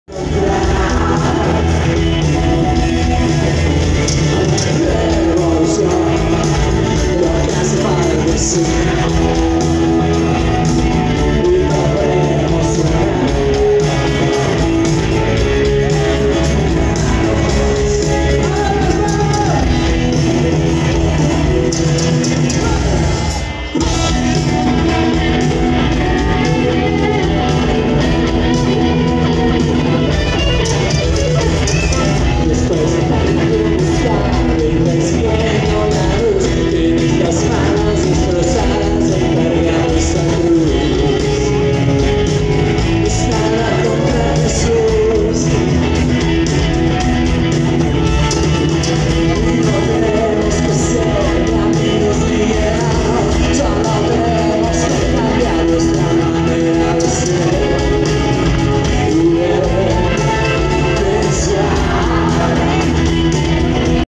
Fobia 2018
Fobia en Auditorio Banamex Monterrey